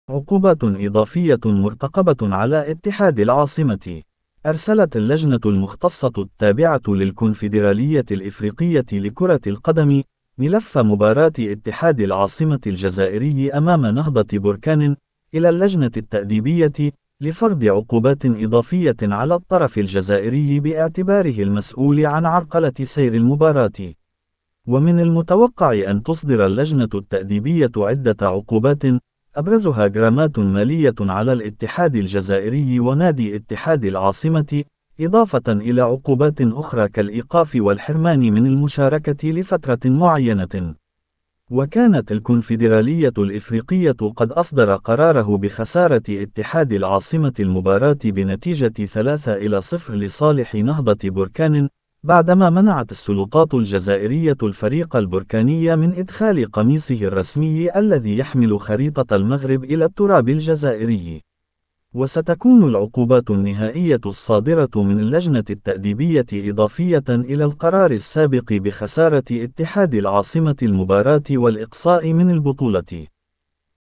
عقوبات إضافية مرتقبة على اتحاد العاصمة
أرسلت اللجنة المختصة التابعة للكونفدرالية الإفريقية لكرة القدم، ملف مباراة اتحاد العاصمة الجزائري أمام نهضة بركان، إلى اللجنة التأديبية، لفرض عقوبات إضافية على الطرف الجزائري باعتباره المسؤول عن عرقلة سير المباراة.